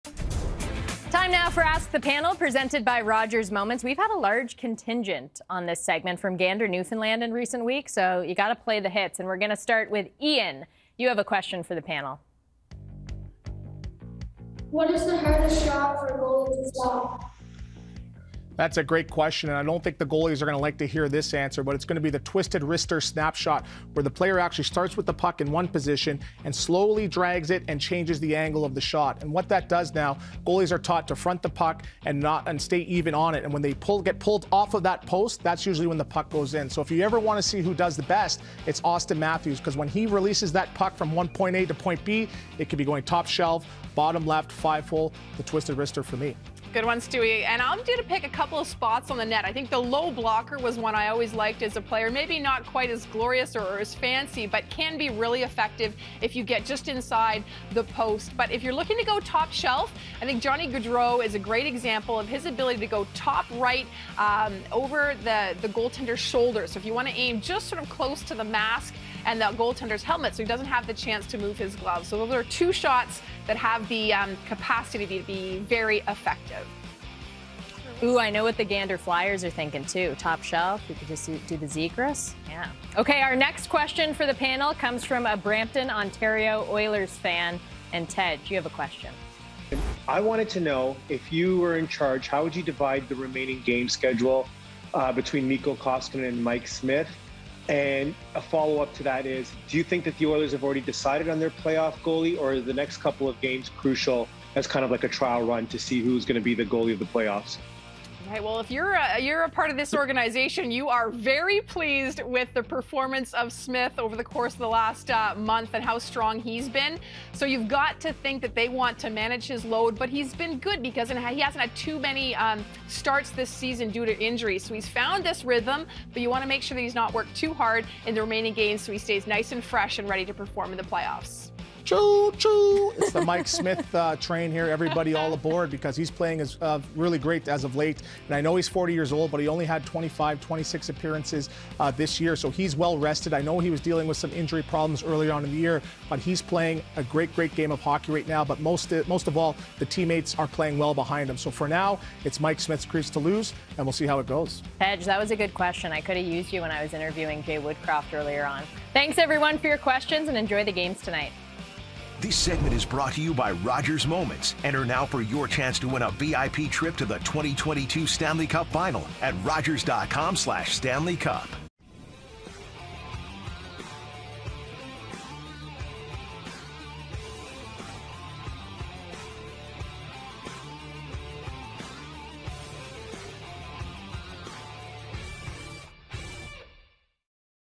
Ask The Panel: What's The Hardest Shot For A Goalie To Stop?
Hockey fans had a chance to 'Ask The Panel' about how the Edmonton Oilers should handle their goalie situation for the remainder of the regular season and what shot goalies have the most trouble stopping.